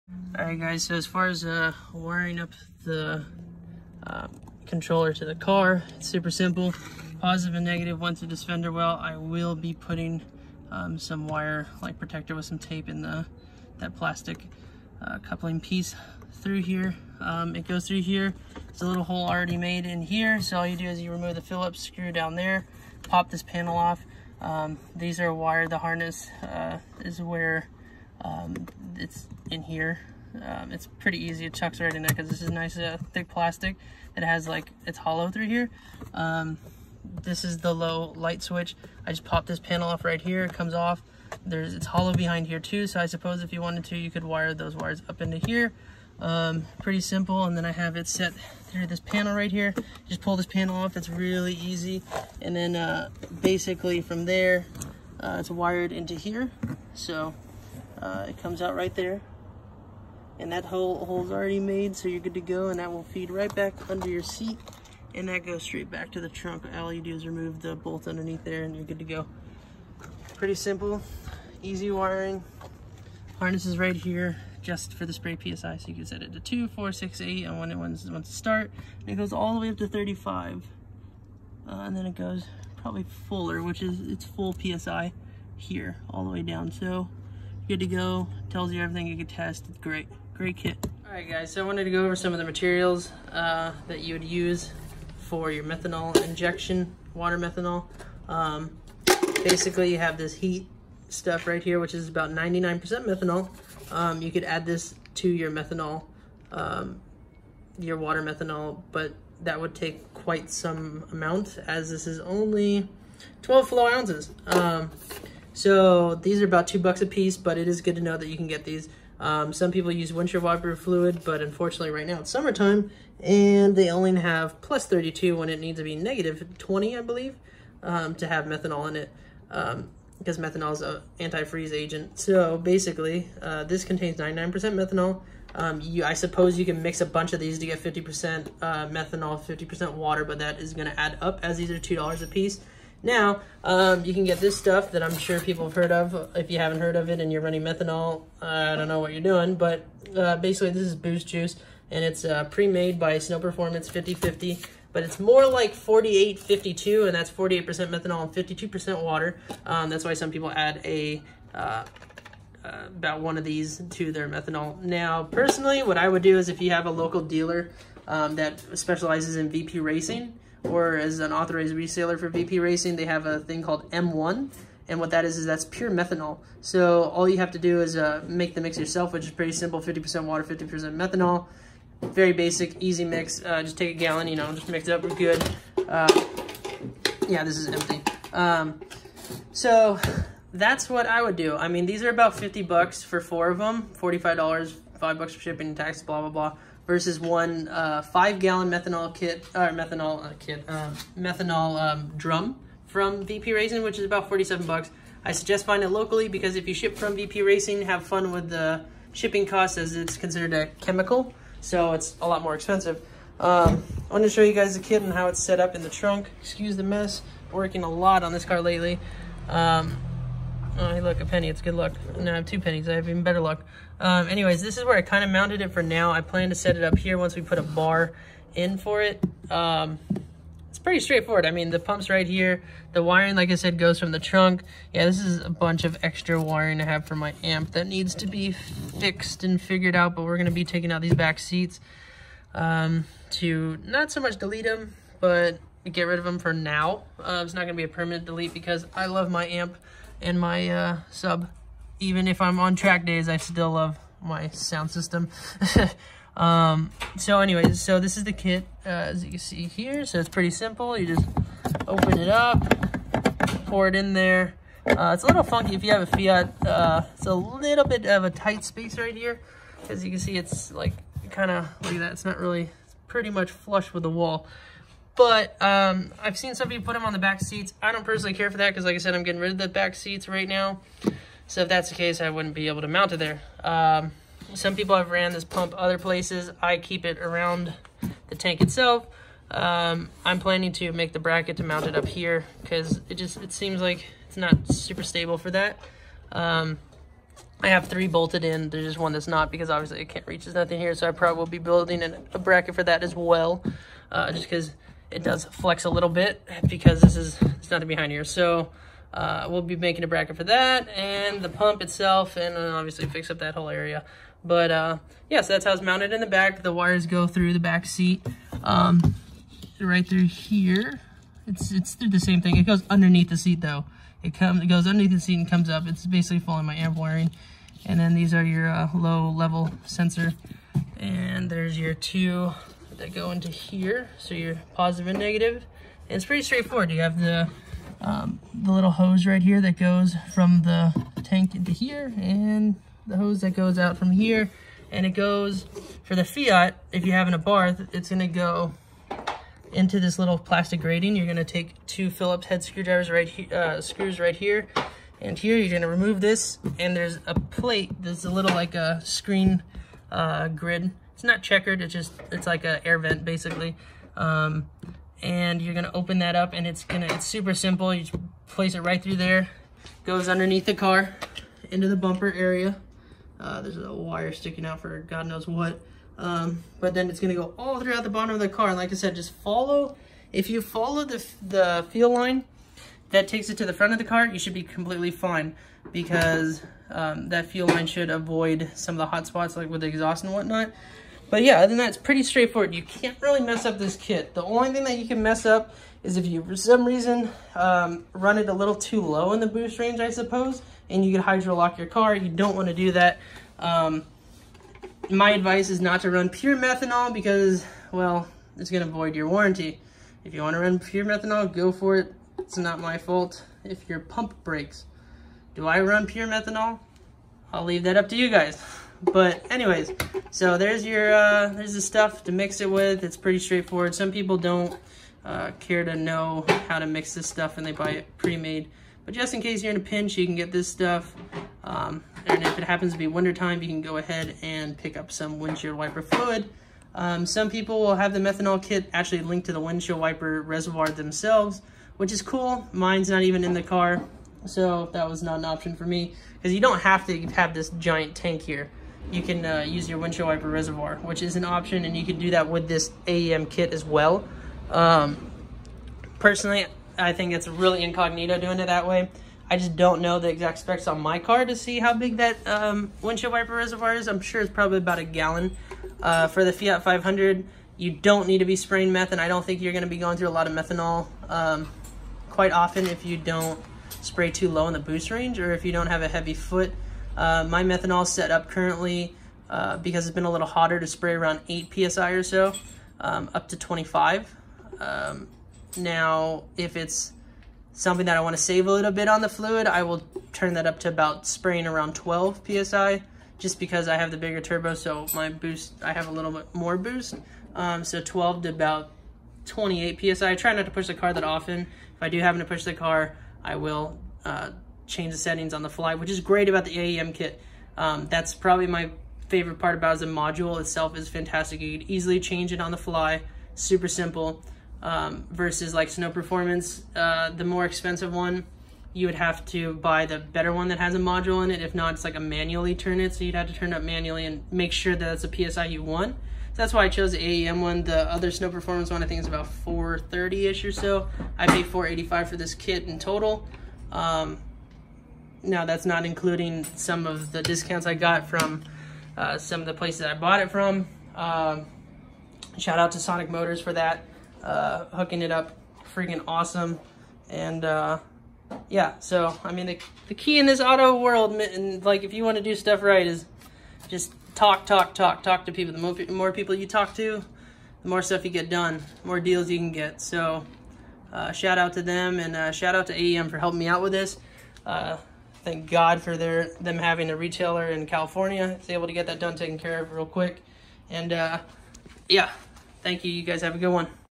Fiat 500 Abarth Methanol Injection Kit Walkthrough Part Two DIY
Hey there Youtube,
   Sorry for the low quality video, my editing software is having some bugs currently and I can't combine the two videos so there is a part one and two. This is a basic walkthrough of an install for water methanol on the abarth. I highly recommend a welded bung for the nozzle port as that will be more reliable. The routing for this is very simple on AEM's kit. I currently run Snow Performance's boost juice which is about a 50 /50 mix. You can also use HEET as a mix as well. The easiest way is with Snow performance's 50/50 mix. You can mix your own with VP racing's M1 5 gallon. It's pretty straight forward, just follow the nozzle size for you car but it comes pre installed with a medium sized nozzle pre installed so you will want to change that out with the smaller of the three options included in the set. The wires connect + & - to the battery and then to the AEM unit / controller, from there you have a couple more wires in which they will be going to the pump and the low fluid sensor as well as an optional led light to show when the nozzle is spraying. Two wires for each, negative and positive. (the pump, the tank, led light, and to the battery).

AEM Kit Part Number: 30-3350

Can be bought on ebay for around $45

Tools needed
Wire cutters, wire connectors, mechanics tool set, patience, zip ties